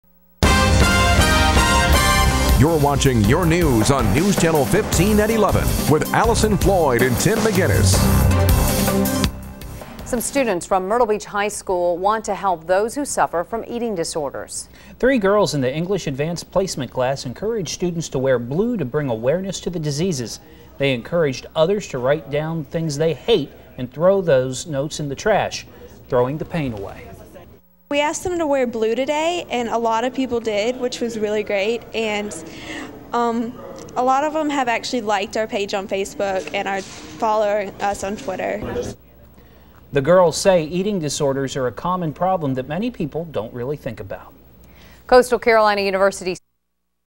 New Beginnings
Myrtle Beach High students raise awareness about eating disorders